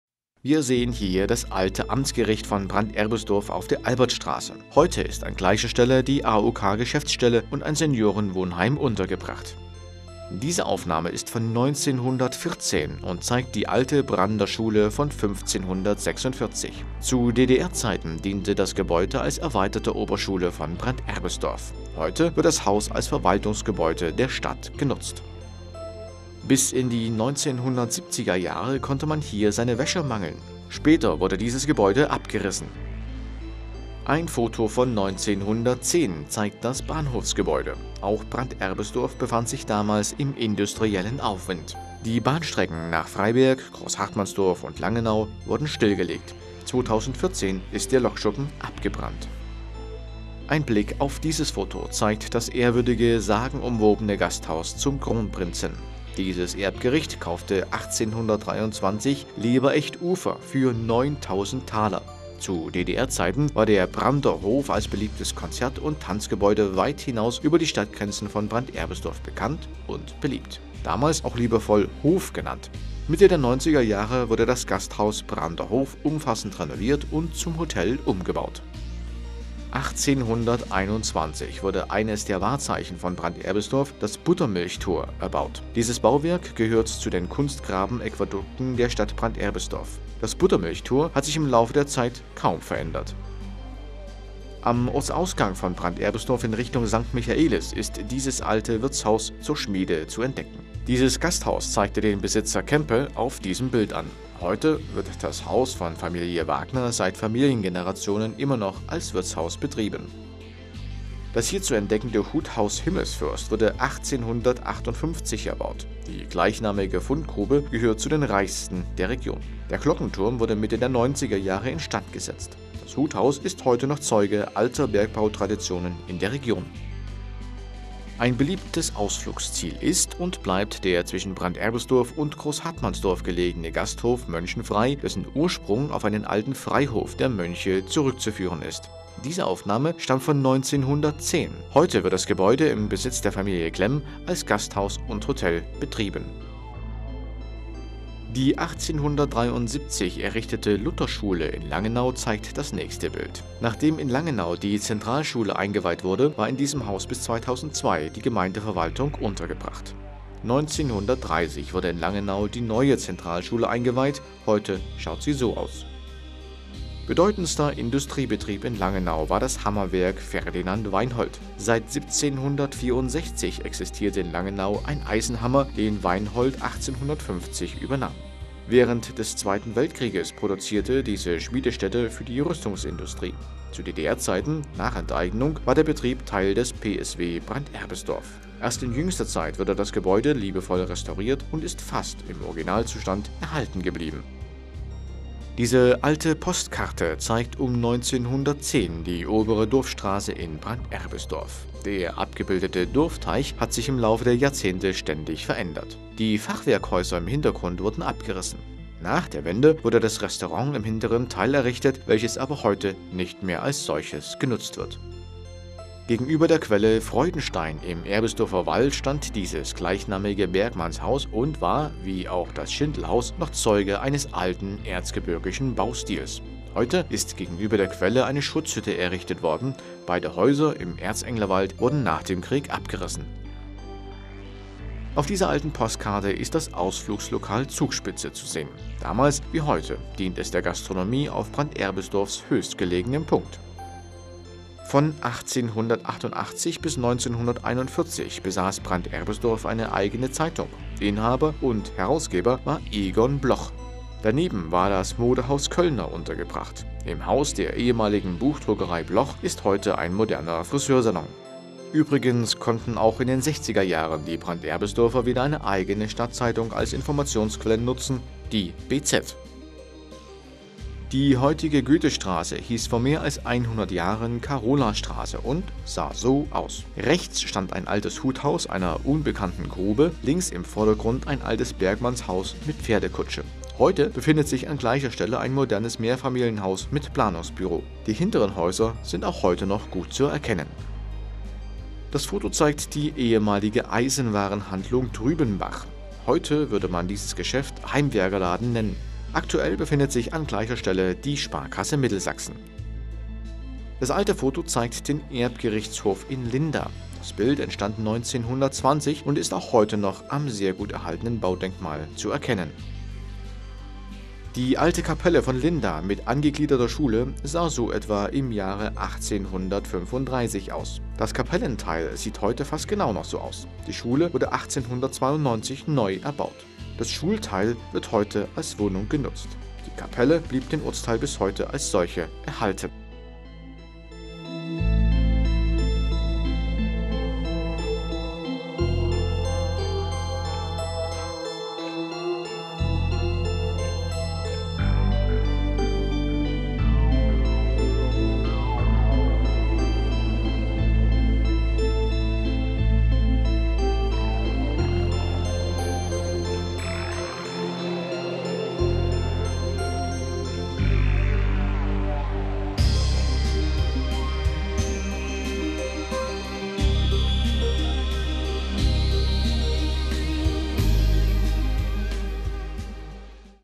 Fotodokumente von “Damals” und “Heute” in Brand-Erbisdorf und Umgebung
Im Beitrag Bilder präsentieren aus einer Zeit, in der das Leben wahrscheinlich in vielerlei Hinsicht schwieriger war, die Menschen aber einfacher und bescheidener.
Gehen Sie mit uns gemeinsam auf eine Zeitreise in die Vergangenheit und vergleichen Sie für uns sichtbare Veränderungen an Hand vieler Fotodokumente von „Damals“ und „Heute“.
„Wir erinnern wir uns, wie es früher war“ – so heißt auch der Titel eines der Bücher, welches wir u. a. als Foto-Quelle für unseren Beitrag verwendet haben.